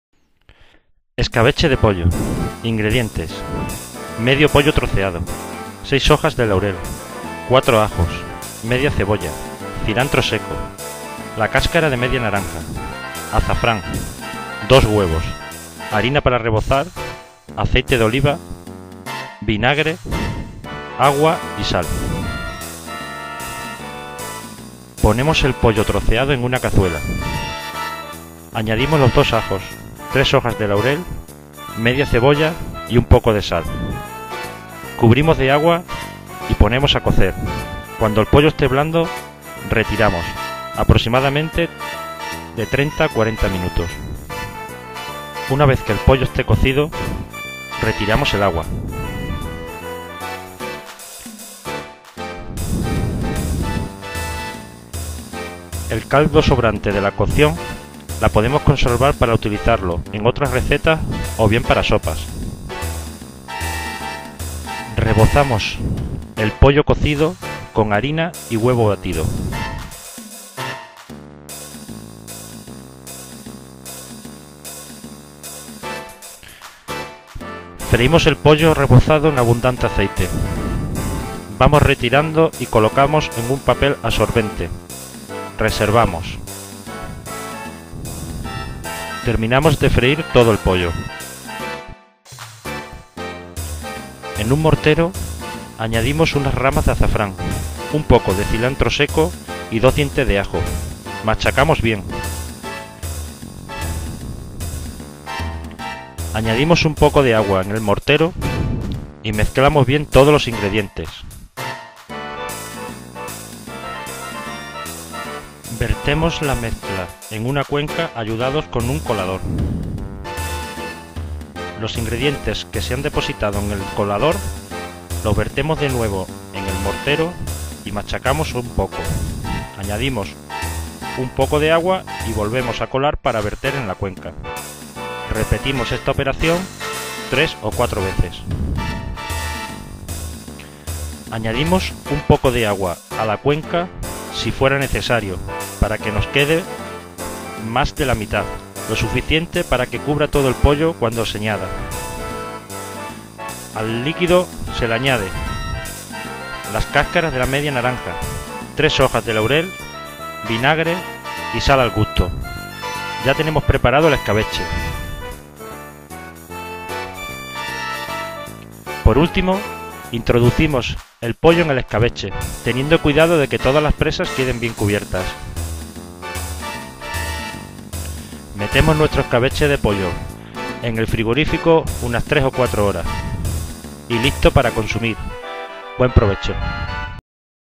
ESCABECHE DE POLLO
Fotos narrada para la elaboración del "ESCABECHE DE POLLO": receta tradicional de Miajadas(Cáceres,Extremadura)